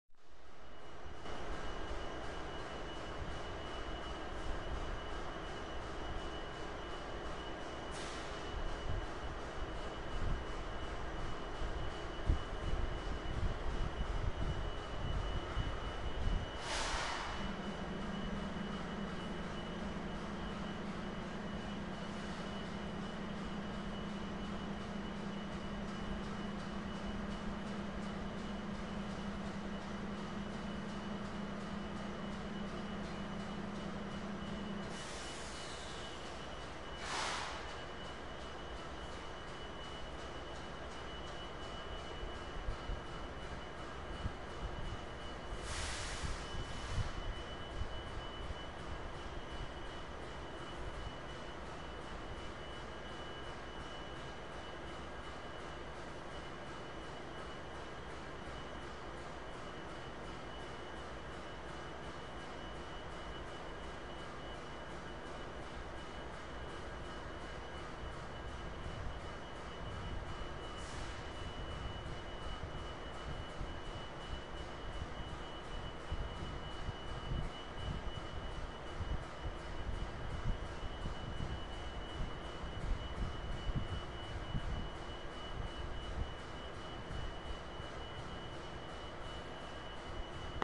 Tight Fit For A High And Wide
High and wide being pulled by an MP15DC and a GenSet. Filmed in July 2011